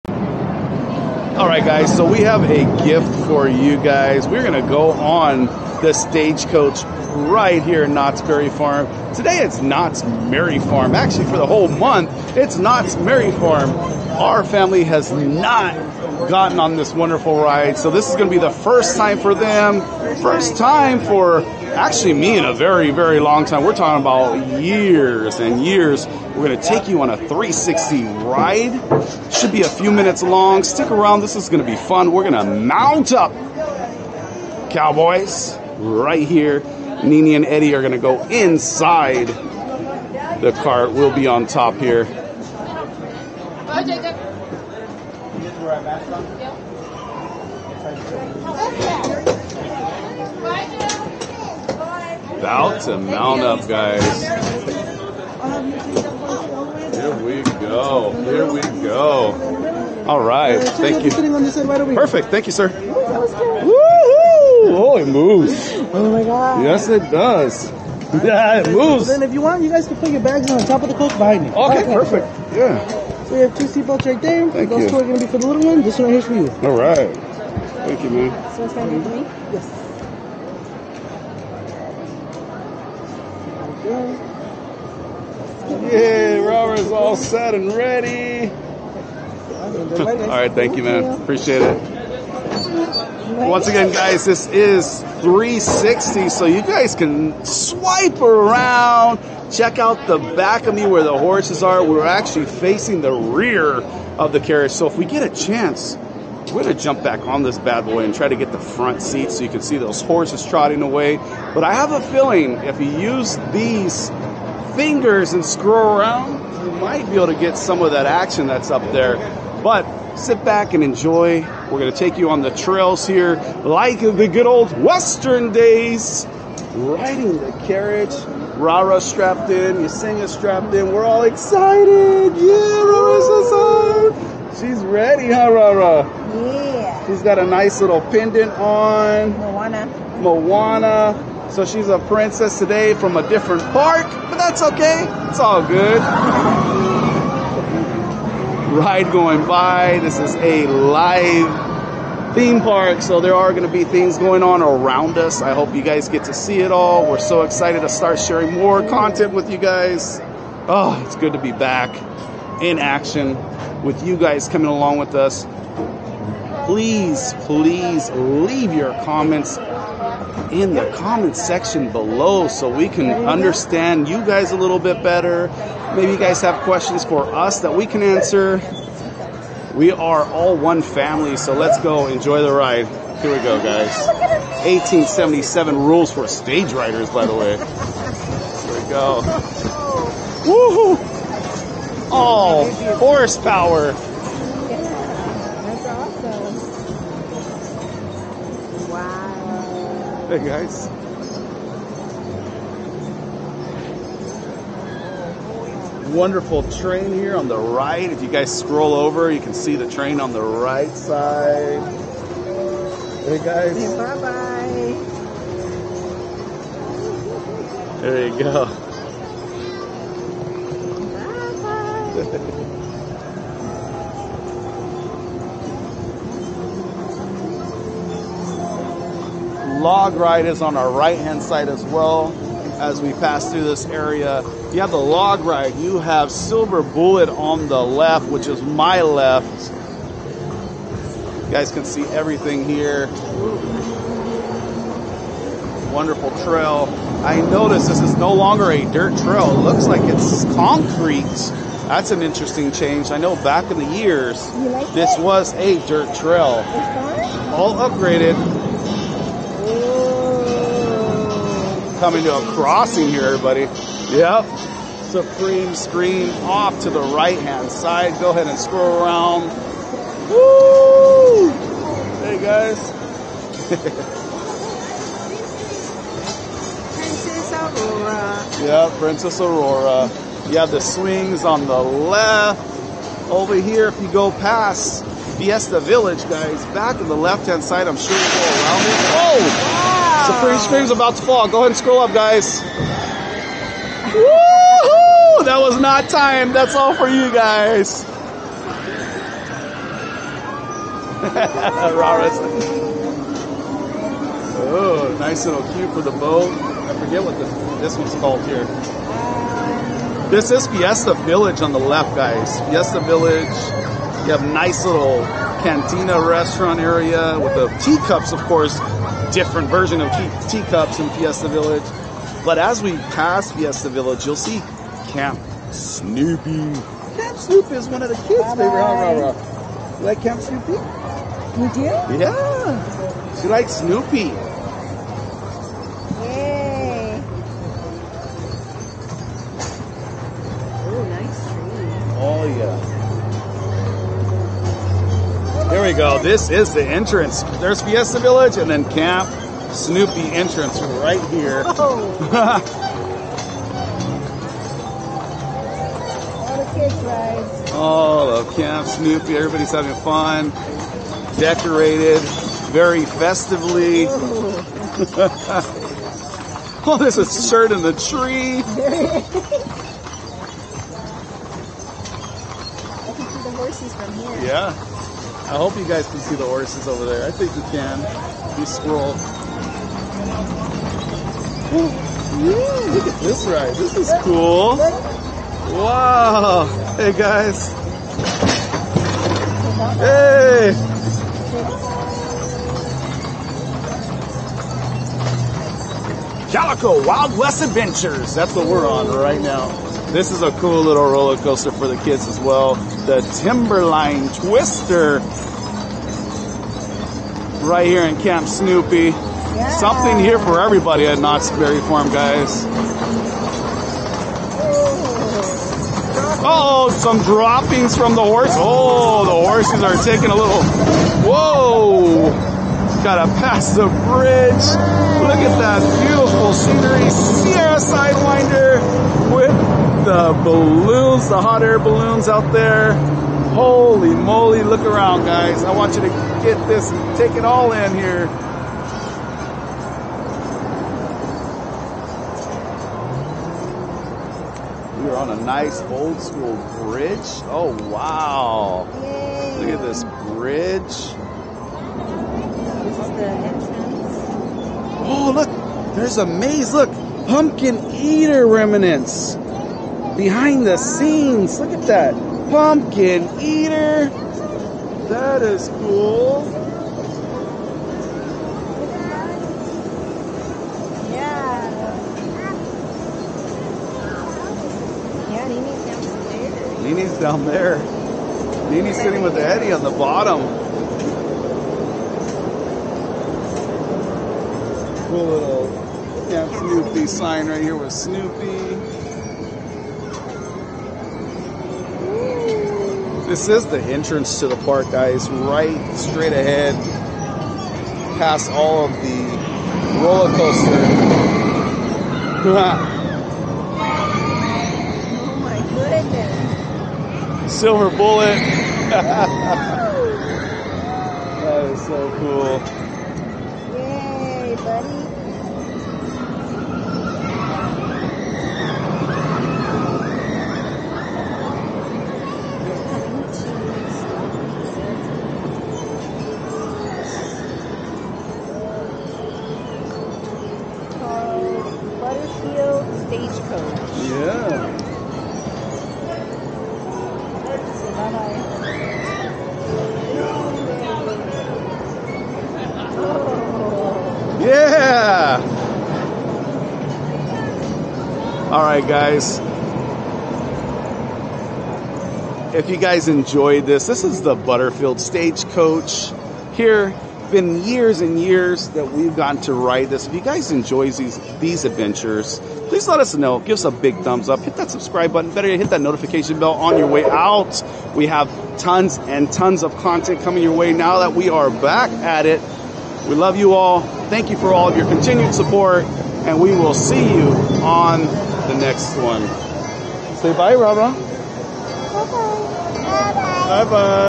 Butterfield Stagecoach | Knott's Berry Farm. A 360 Ride Experience and Tour of the Trail. Knott's.
Butterfield Stagecoach | Knott's Berry Farm. A 360 Ride Experience and Tour of the Trail. Knott's Merry Farm Surprise. We take you on a 360 ride on the Butterfield Stagecoach.

All contributions are greatly appreciated.